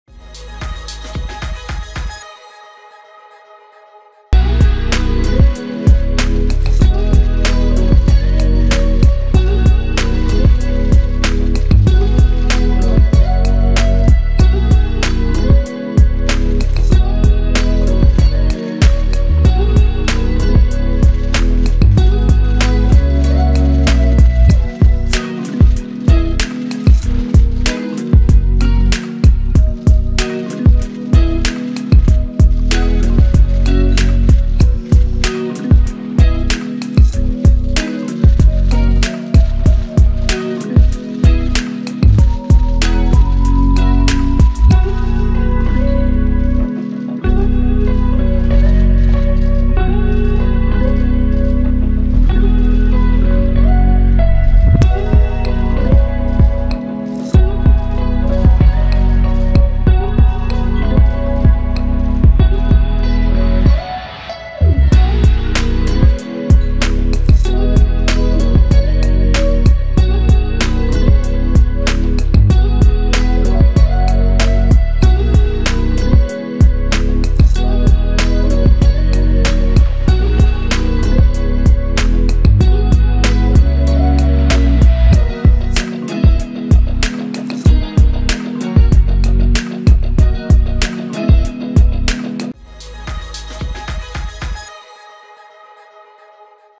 [4K] 박성훈(Park Sunghoon), 말하지 않아도 '볼하트까지 척척~' 사실 귀여운 사람입니다 (‘버버리’ 리뉴얼 오픈 포토콜 Phototime)
[티브이데일리 채아영 기자] 배우 박성훈이 31일 오전 서울 서초구 신세계백화점에서 열린 패션 브랜드 리뉴얼 기념 포토콜에 참석해 포즈를 취하고 있다.